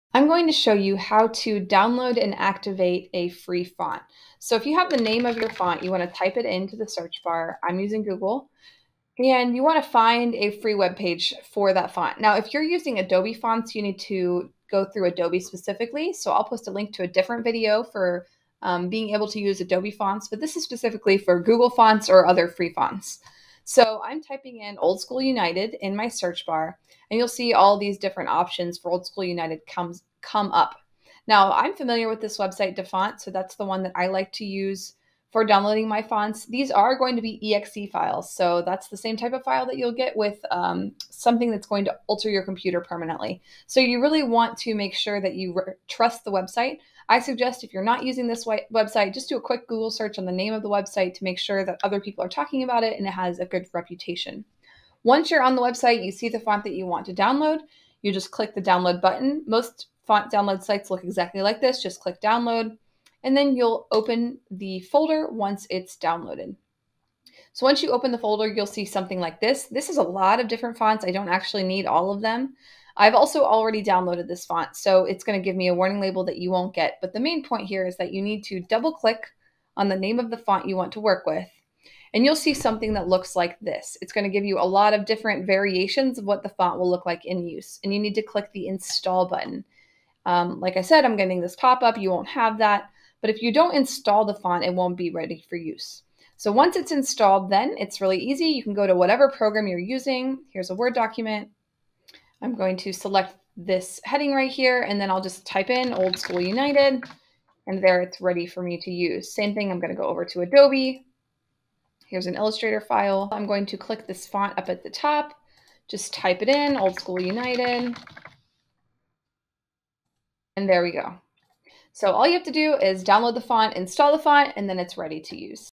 How to activate and use FREE FONTS (Google fonts, etc.)
Learn how to safely download, install and use free fonts online.